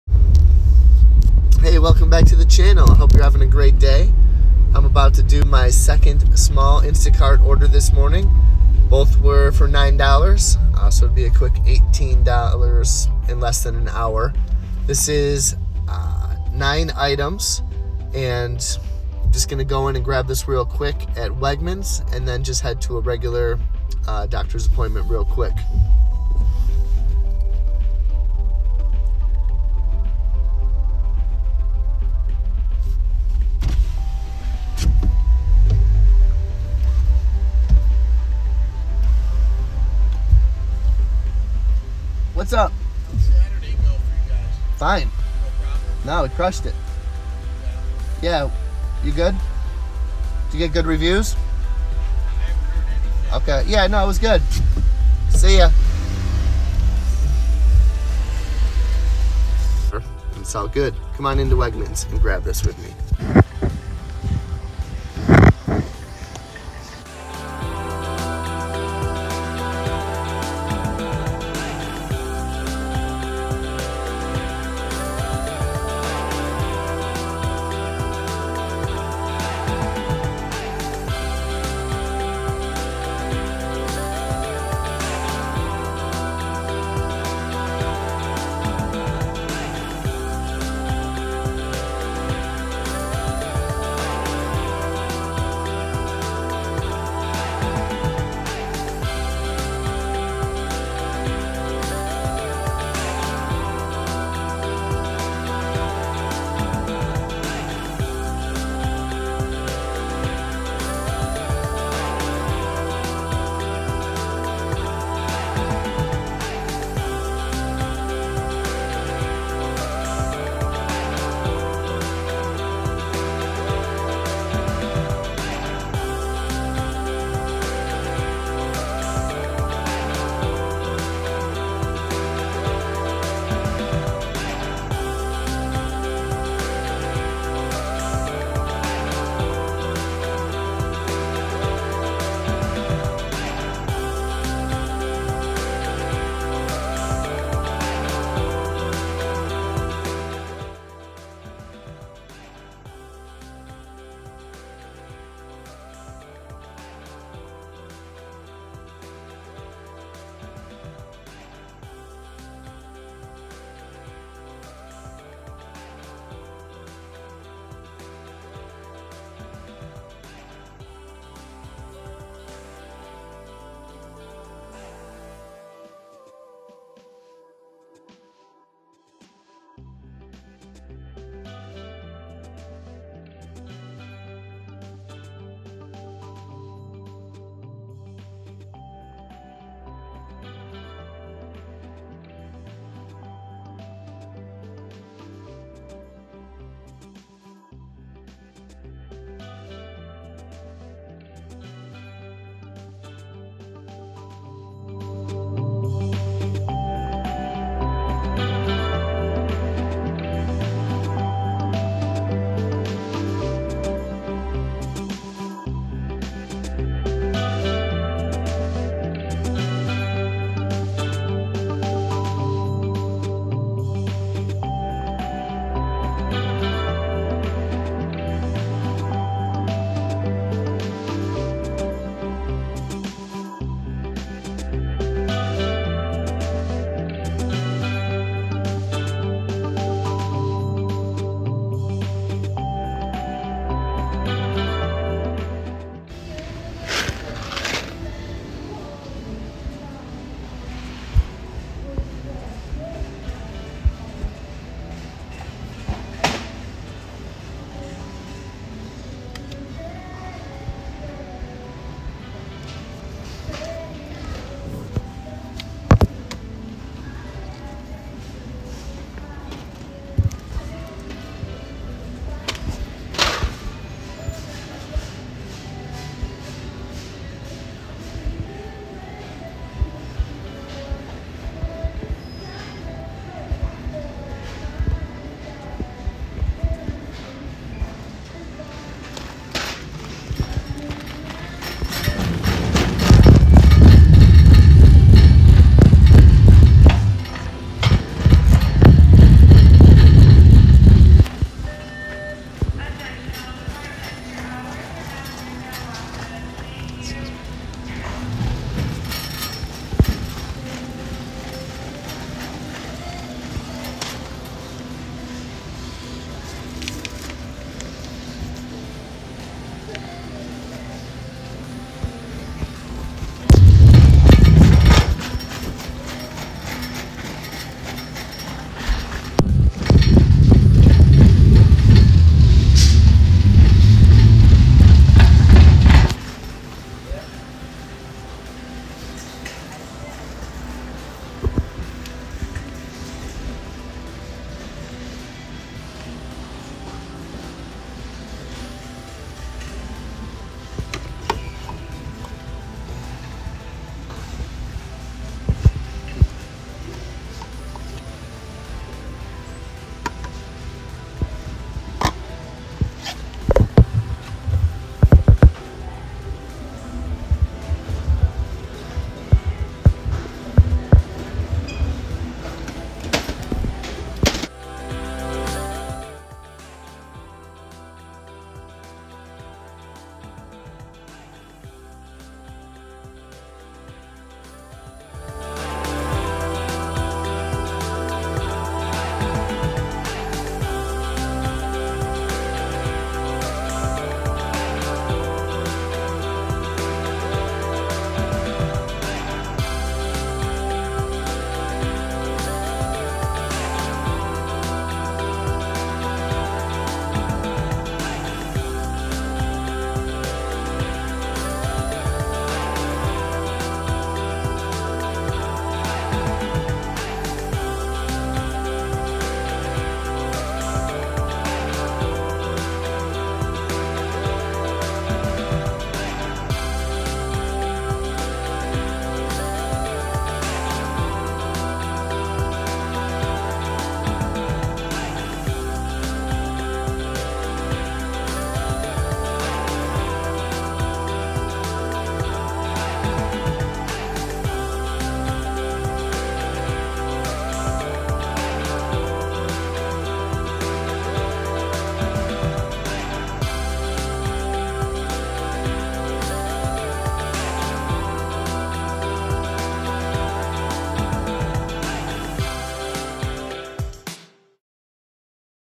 Instacart Shop A Quick & Easy Order With Me
I this video I take you along while I shop a quick and easy order at Wegmans.

Small orders are nice and they get the ball rolling!

I can’t believe I get paid for this!

Instacart Side hustle for your freedom.

MAKE AT LEAST $450 + TIPS if you complete 30 batches in 30 days.

Sign up with Instacart with my code JBARRETTE14133